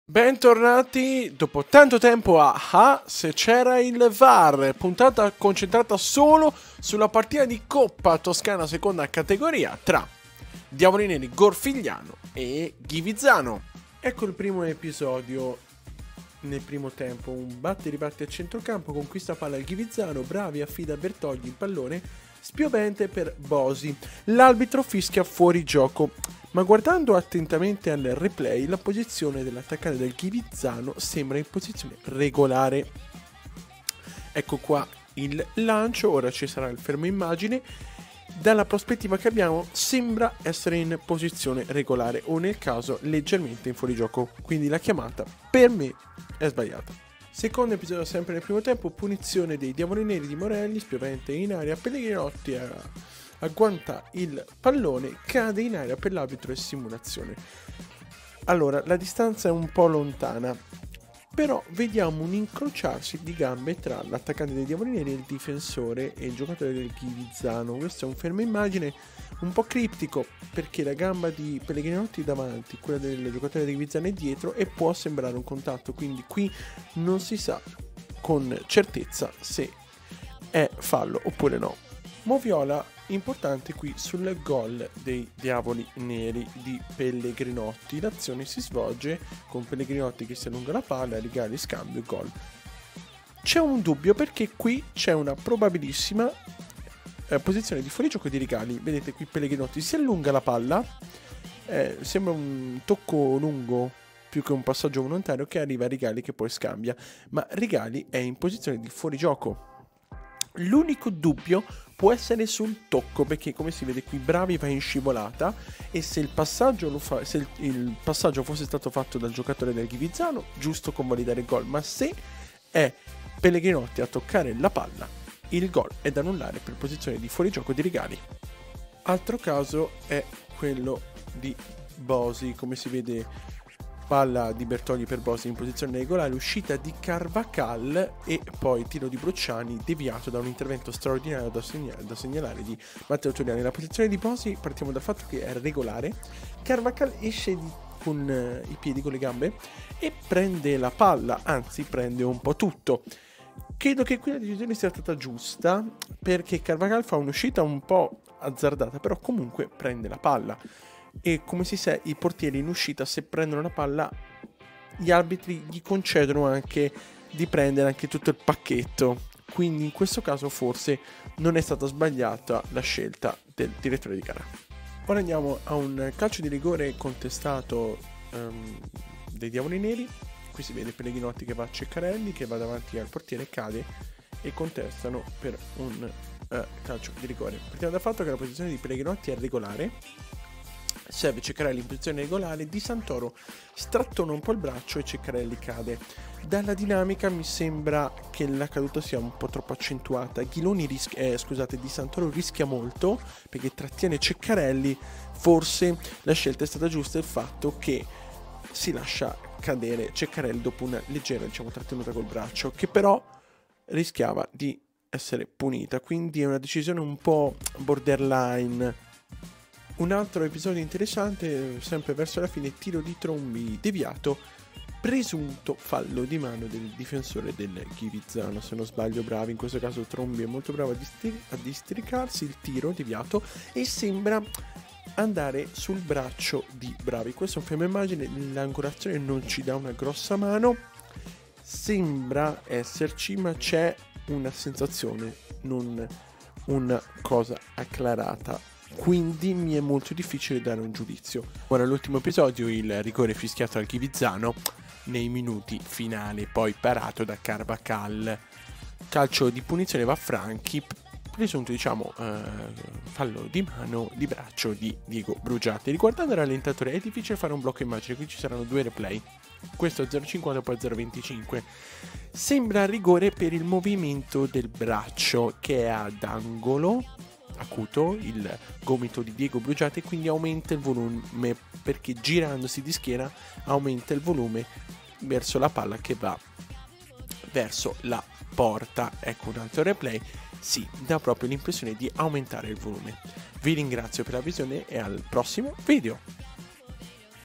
GFC presenta: AHH...SE C'ERA IL VAR - Puntata 10
Casi da moviola della seguente partita:
Diavoli Neri Gorfigliano vs Ghivizzano

Regia e commento Gian Marco Bertei